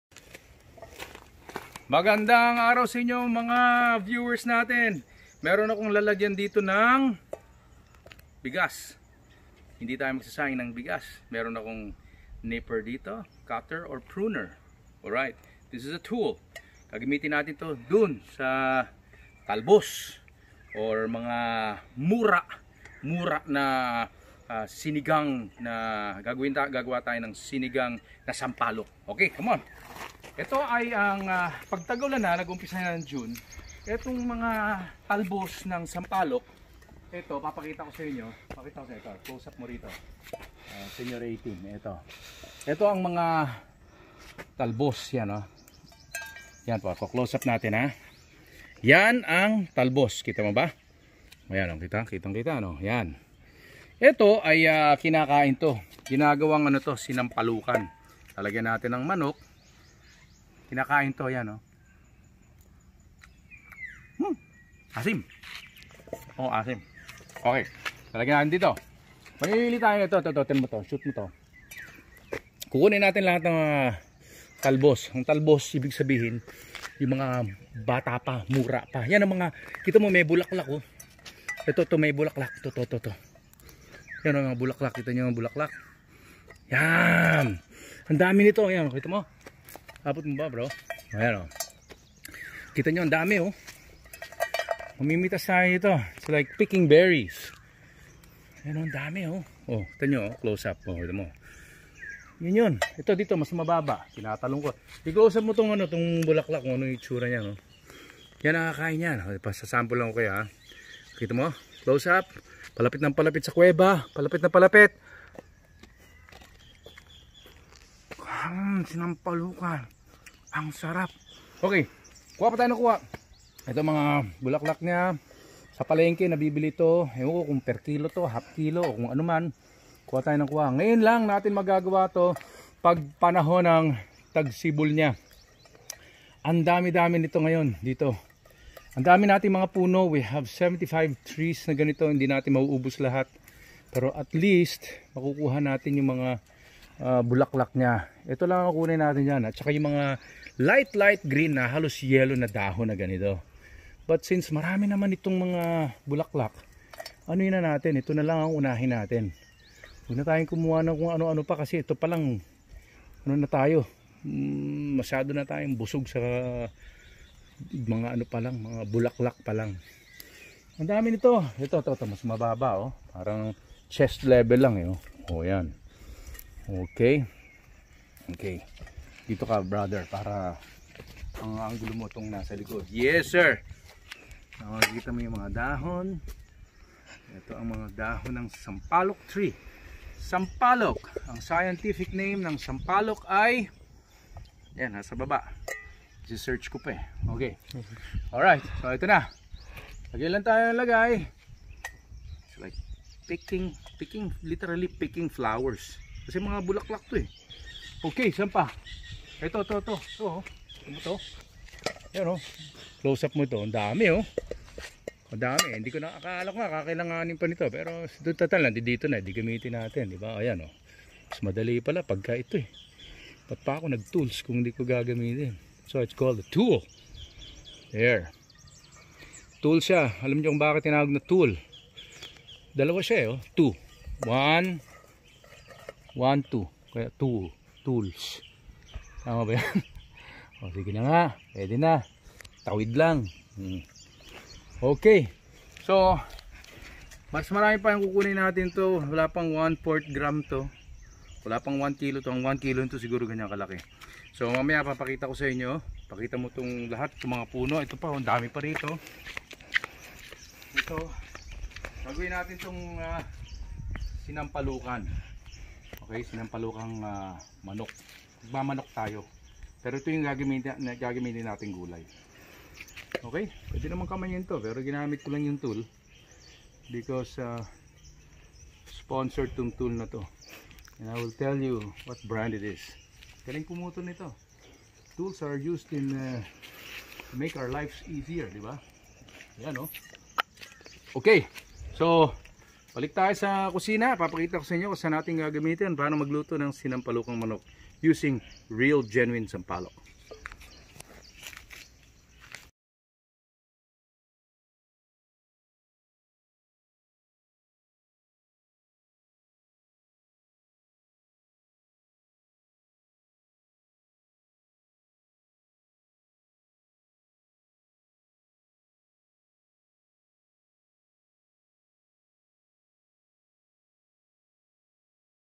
Sinampalukang Manok part 1
Picking flowers from Tamarind Tree to make a dish called, SINAMPALUKAN